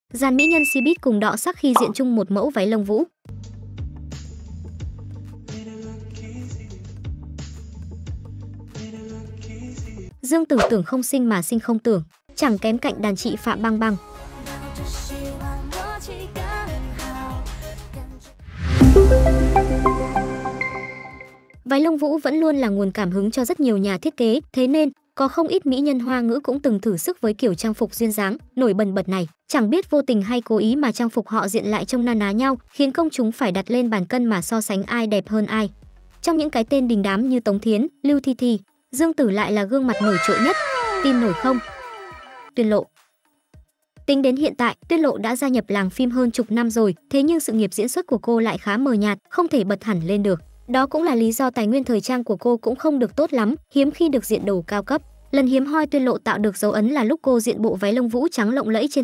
Dàn mỹ nhân Cbiz cùng đọ sắc khi diện chung một mẫu váy lông vũ: Dương Tử xinh không tưởng
Váy lông vũ vẫn luôn là nguồn cảm hứng cho rất nhiều nhà thiết kế, thế nên, có không ít mỹ nhân Hoa ngữ cũng từng thử sức với kiểu trang phục duyên dáng, nổi bần bật này. Chẳng biết vô tình hay cố ý mà trang phục họ diện lại trông na ná nhau, khiến công chúng phải đặt lên bàn cân mà so sánh ai đẹp hơn ai. Trong những cái tên đình đám như Tống Thiến, Lưu Thi Thi... Dương Tử lại là gương mặt nổi trội nhất. Tin nổi không?